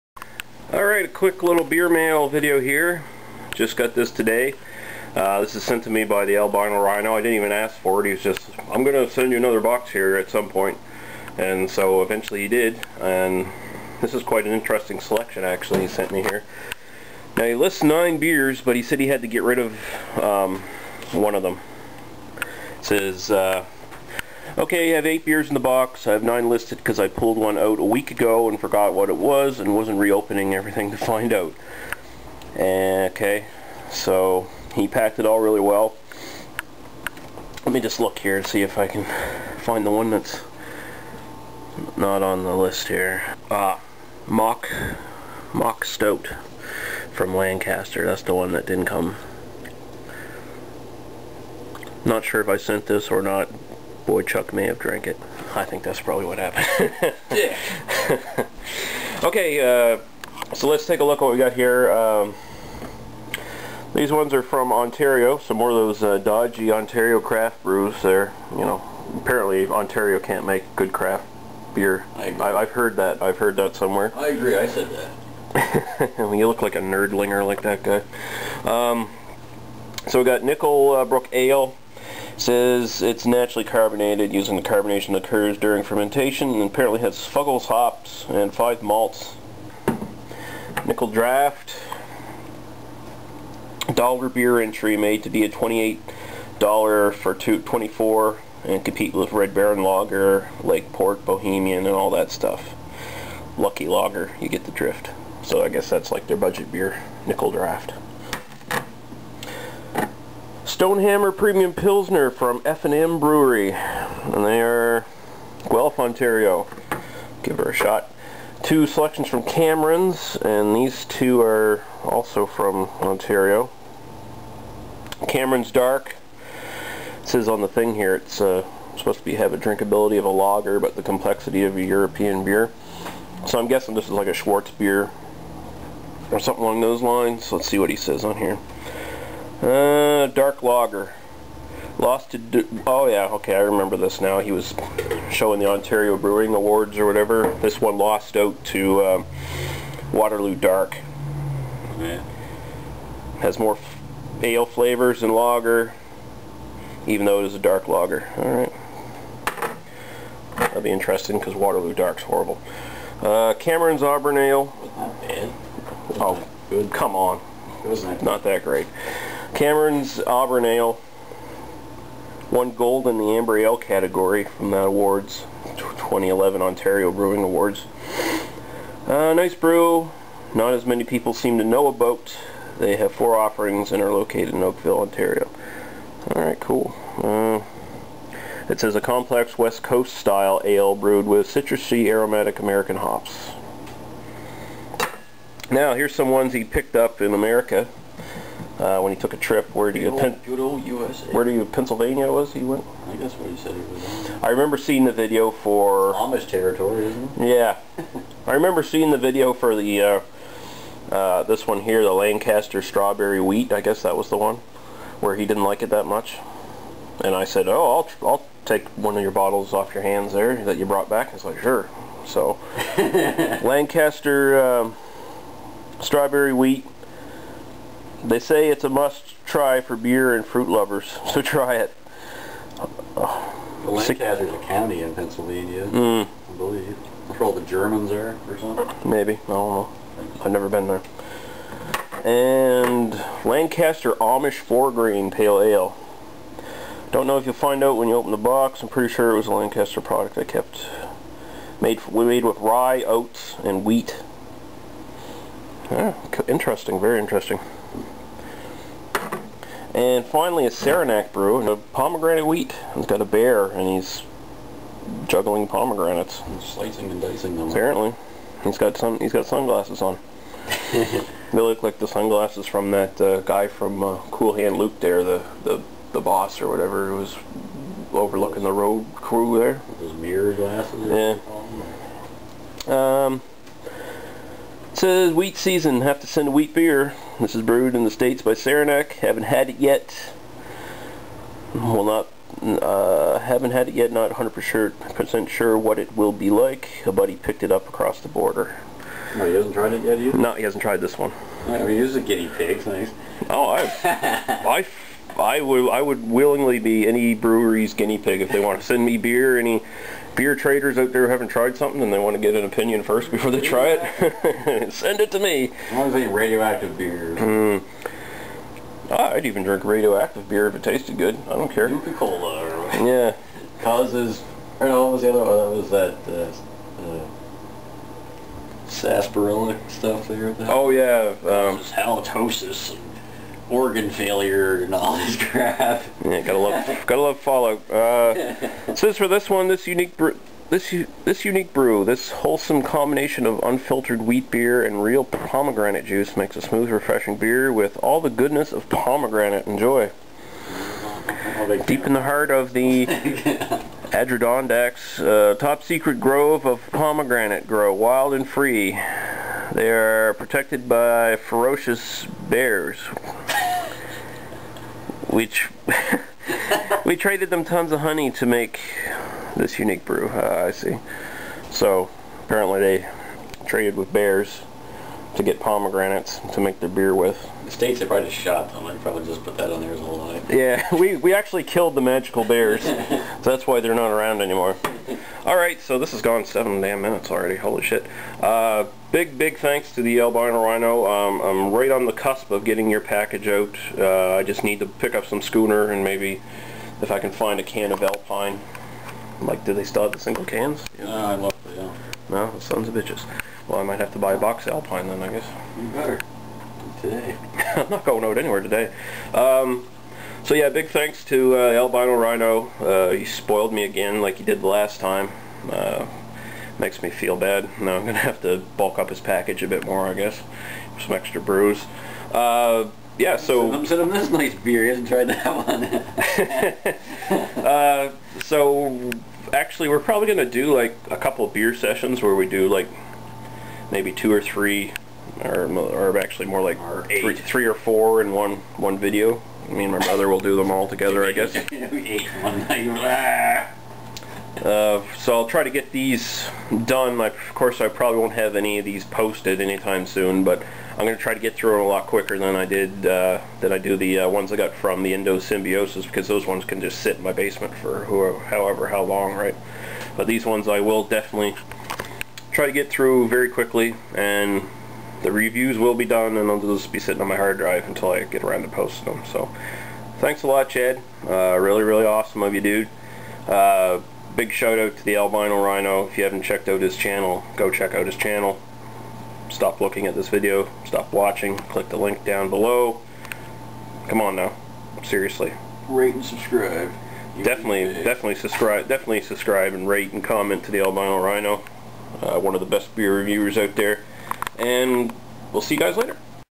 Vlog -- July 25th, 2011: Beer Mail From the Albino Rhino.
A look at yet more interesting beer you're going to have to wait on a bit for the reviews, due to my summer break. All of these were sent to me by the one and only Albino Rhino

Big thanks to him for this awesome beer mail.